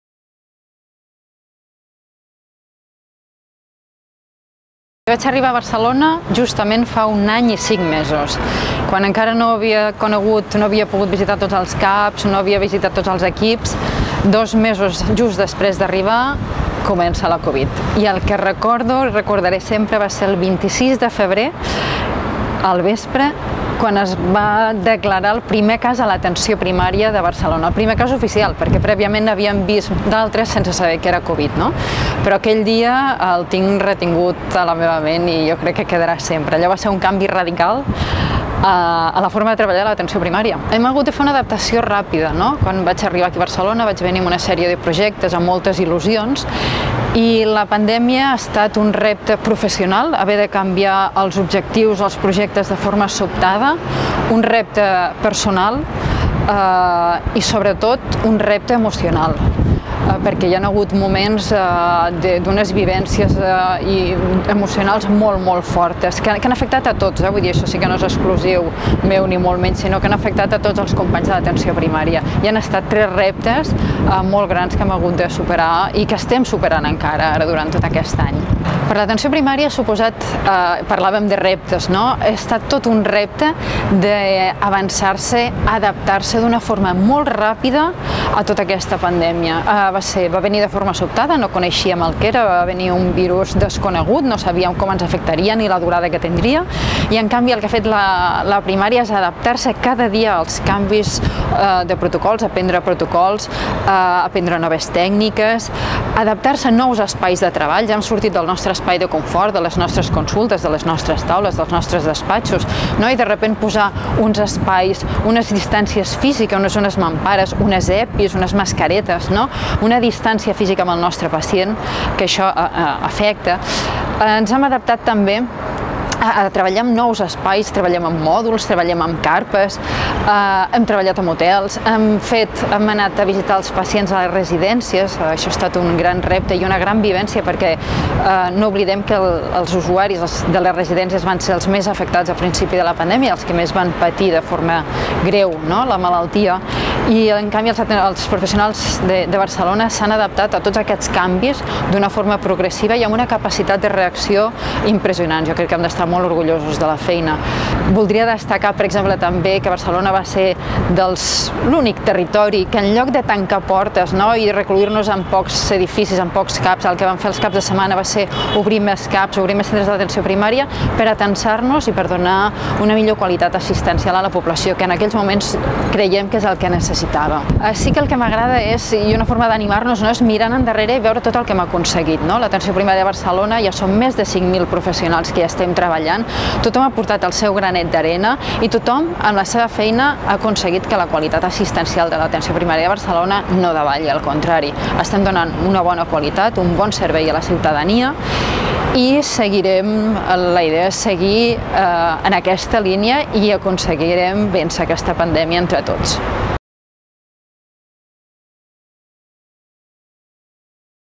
Un any de pandèmia a l’Atenció Primària Barcelona Ciutat de l’ICS
En el Dia Mundial de l'Atenció Primària volem fer una mirada enrere per reconèixer i agrair tota la feina assolida; noves tasques, nous perfils professionals, estudis de recerca i un llarg etcètera que hem recollit amb un resum de les dades més destacades, fotografies del dia a dia, a

Ja ha passat més d’un any des de l’inici de la pandèmia de la covid-19, un any en què l’Atenció Primària Barcelona Ciutat (APBCN) ha demostrat que en equip podem assolir tots els reptes que se’ns plantegin.

Volem fer una mirada enrere per reconèixer i agrair tota la feina assolida; noves tasques, nous perfils professionals, estudis de recerca i un llarg etcètera que hem recollit amb un resum de les dades més destacades, fotografies del dia a dia, aparicions als mitjans de comunicació i altres accions comunicatives que han donat visibilitat el testimoni de l’experiència viscuda.

Som un gran equip, Som primària!